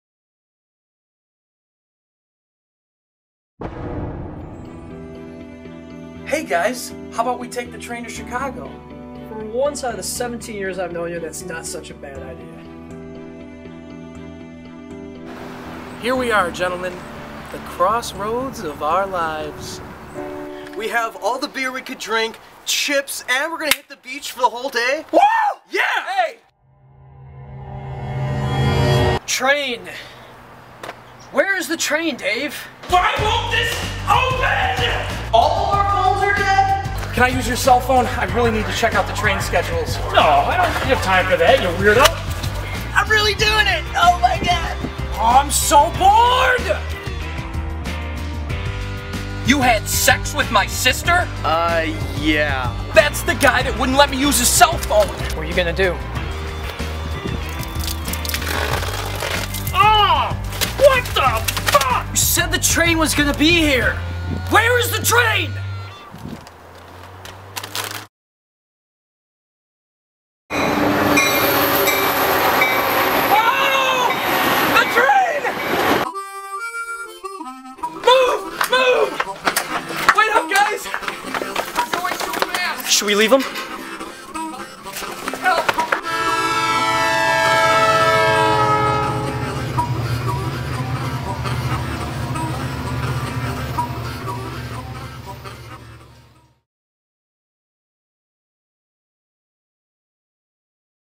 The Departure - A Metra Story HD
It's a trailer, to a movie, that will never get made. ENJOY!